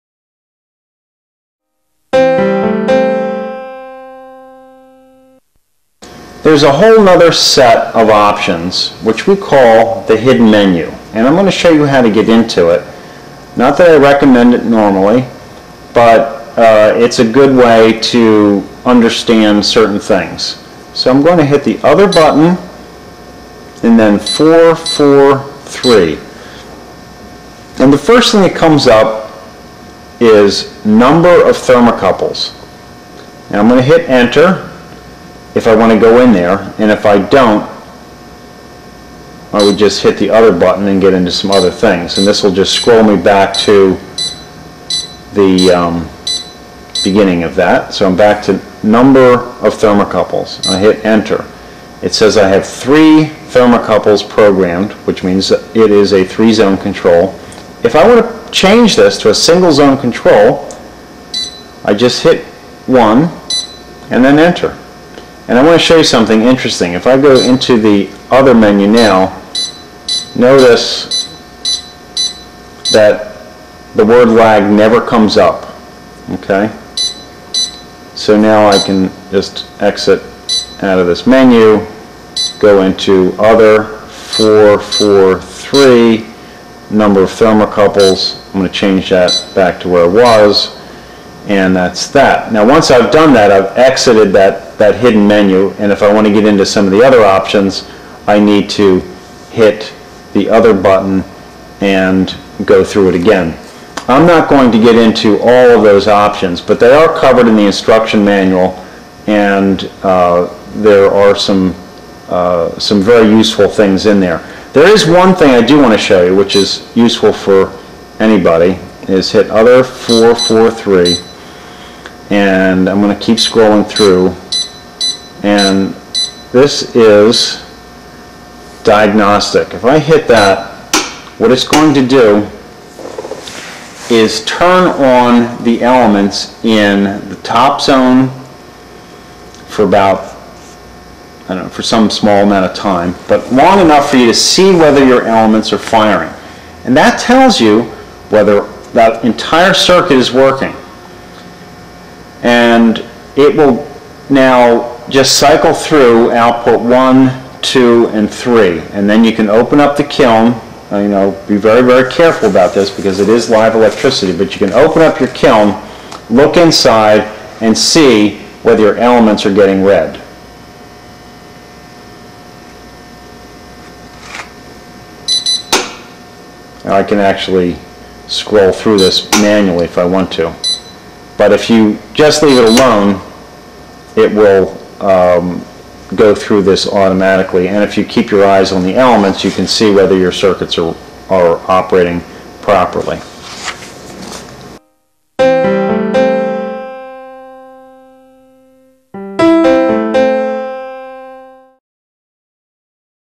Setting other options on the DynaTrol Control on an L&L kiln
How to set other more complicated options on an L&L DynaTrol program control on L&L electric ceramic pottery kilns.